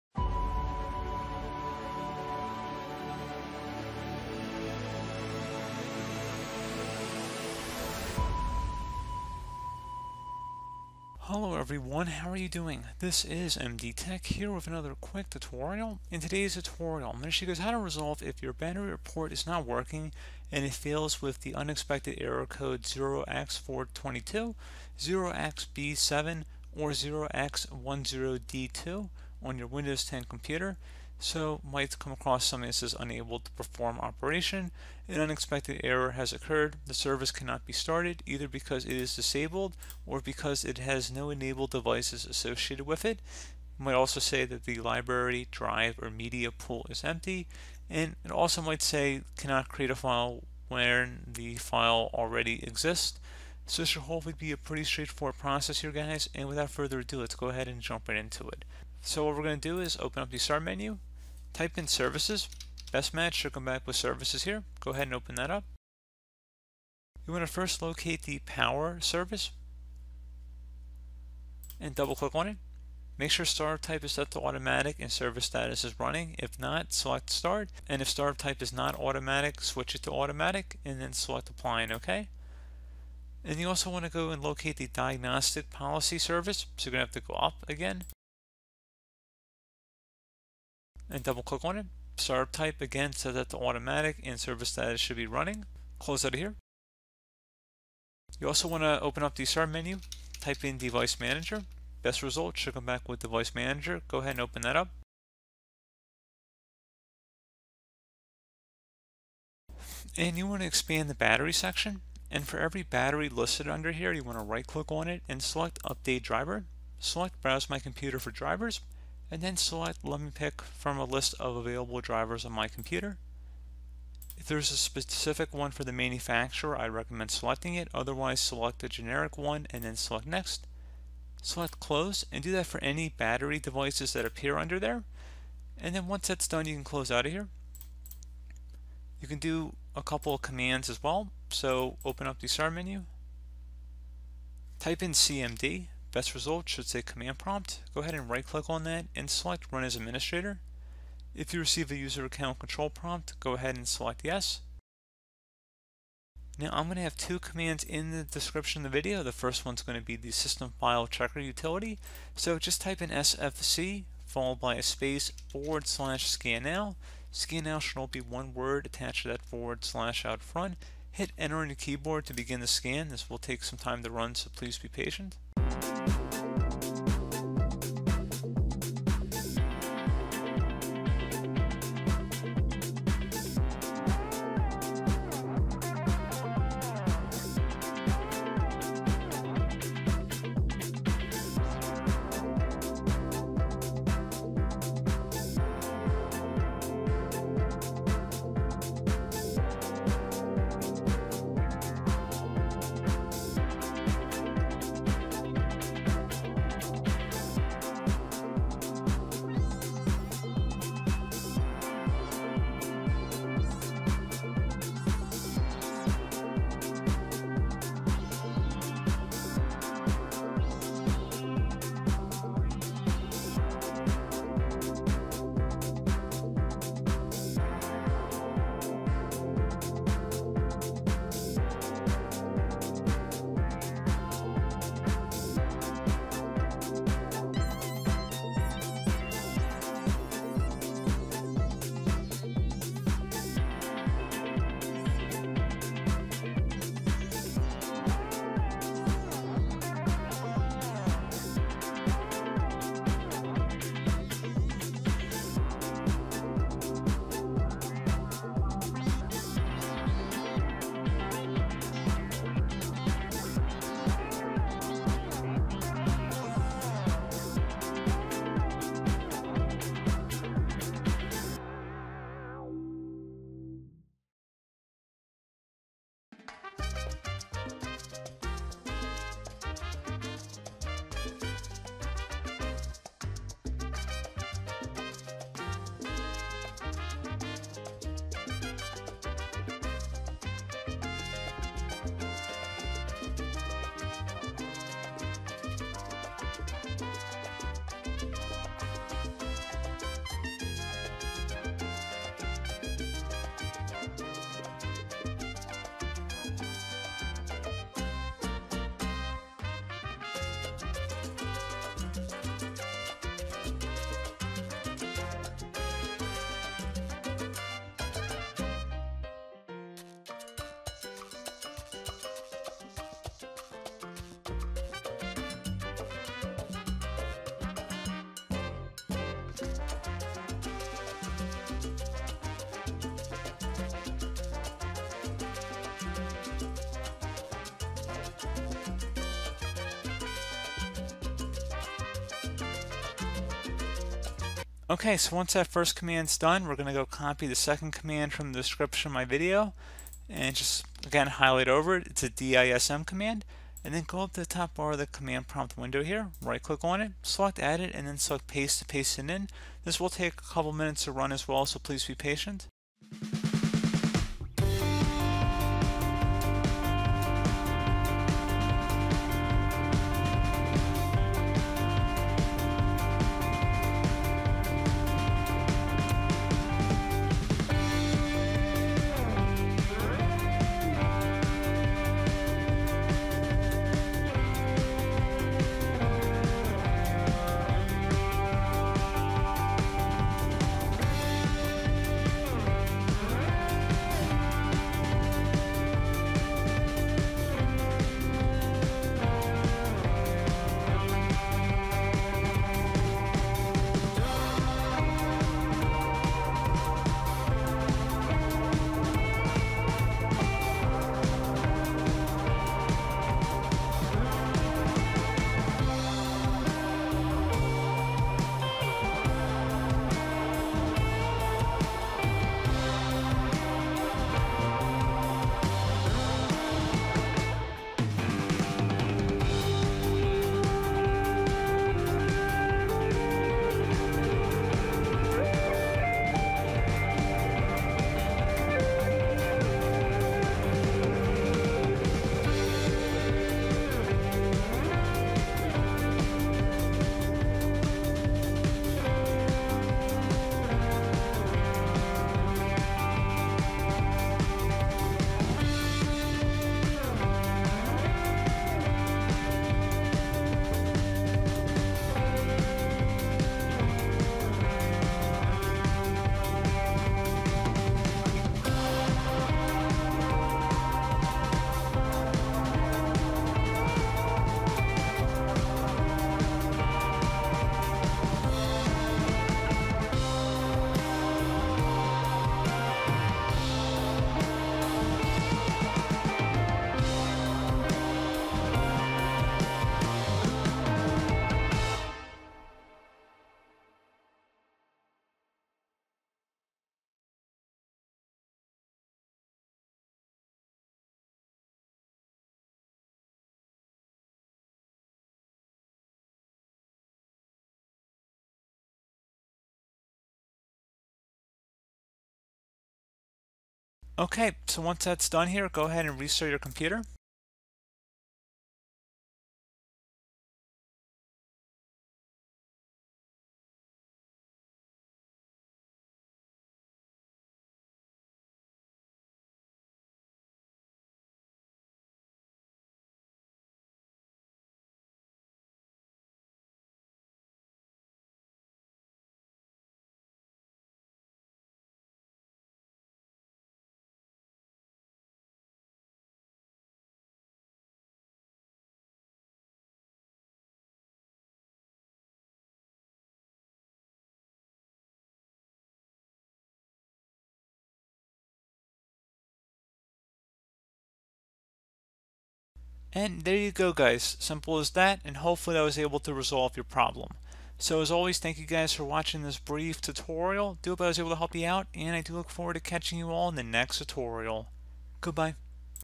Battery Report Not Working, Fails with Unexpected Errors 0x422, 0xb7 or 0x10d2 on Windows 10 FIX
BatteryReport Not Working, Fails with Unexpected Errors 0x422, 0xb7 or 0x10d2 on Windows 10 FIX.

Commands Used:
sfc /scannow

If when you attempt to generate a battery health report using powercfg /batteryreport command on your Windows 10 system, you encounter either error 0x422, 0xb7 or 0x10d2, then this tutorial is intended to help you.

Issues addressed in this tutorial:
powercfg /batteryreport not working